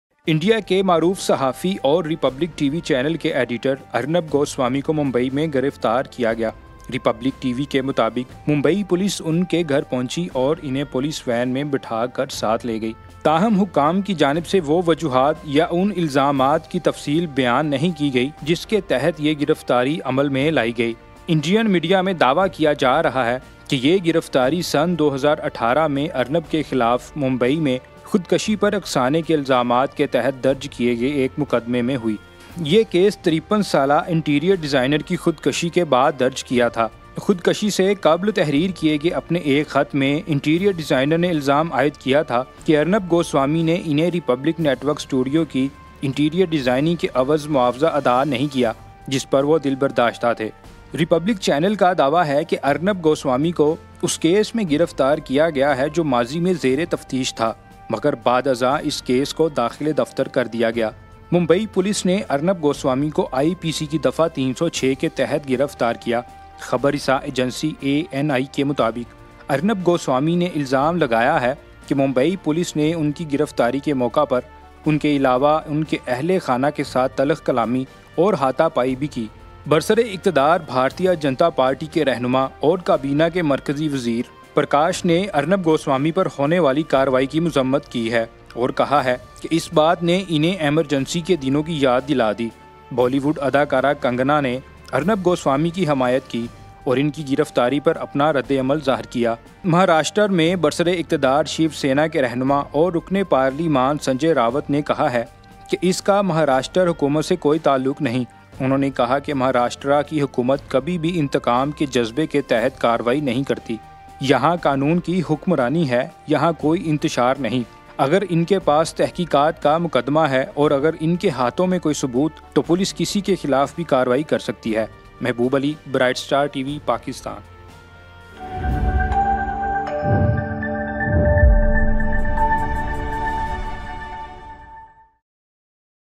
Why Arnab Goswami arrested by Mumbai police
Republic TV Editor-in-Chief Arnab Goswami was on Wednesday arrested for allegedly abetting the suicide of a 53-year-old interior designer in 2018, a police official said.
The official said a team of Alibaug police picked up Goswami from his Lower Parel house in the morning. Goswami, who was seen being pushed into the police van, claimed he was assaulted by police at his home, before being taken away.